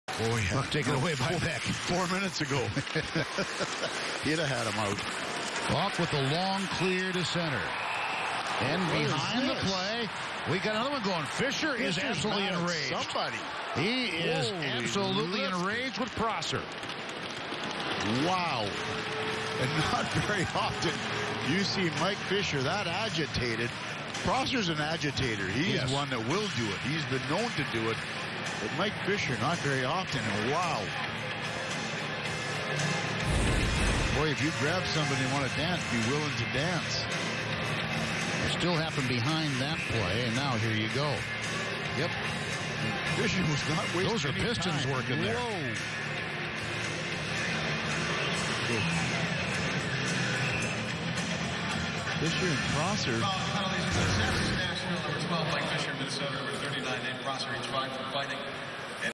Mike Fisher vs Nate Prosser Jan 10, 2015
Mike Fisher vs Nate Prosser from the Nashville Predators at Minnesota Wild game on Jan 10, 2015.